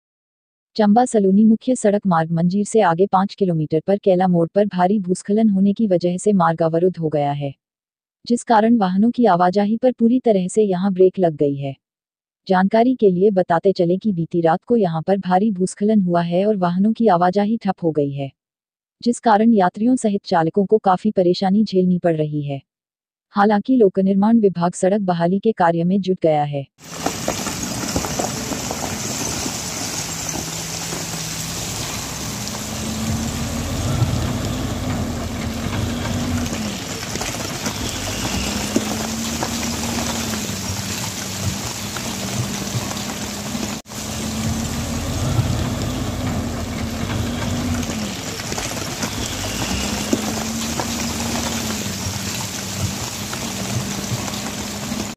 मंजीर-सलूणी मुख्य सड़क कैला मोड़ पर भारी भूस्खलन से अवरुद्ध।
Himachal#Chamba